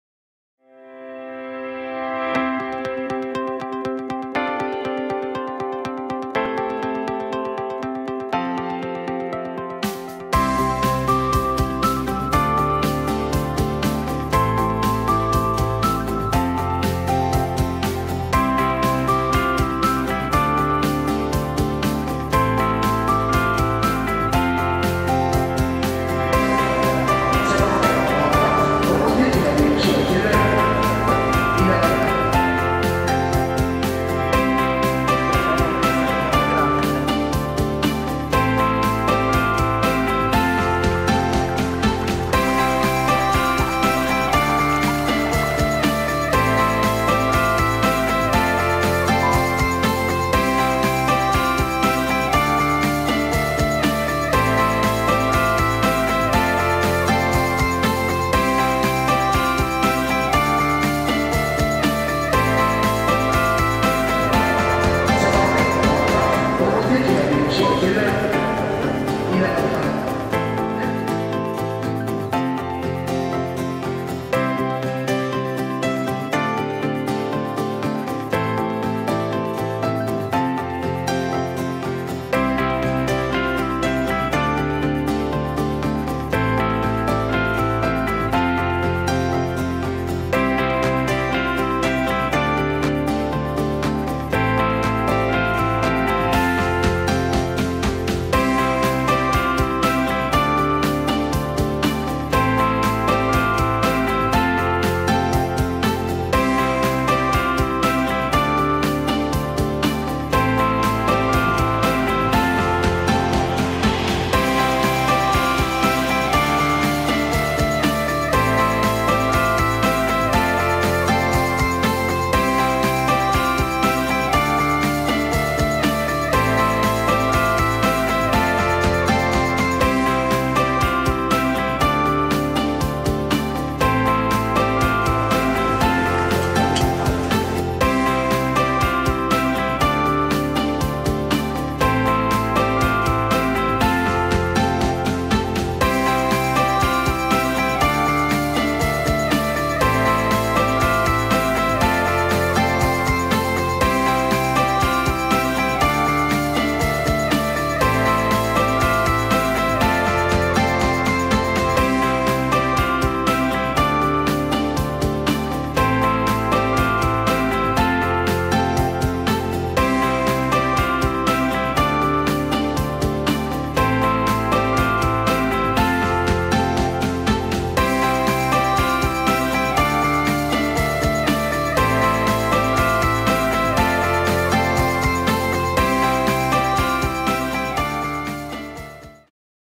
Inside the world's Largest Airport: King Fahd International Airport in Saudi Arabia.
King Fahd International Airport is the largest airport in the world. Also known as Dammam International Airport. It covers approximately 776 square kilometers (300 square miles). The airport can accommodate over 9 million people at a go.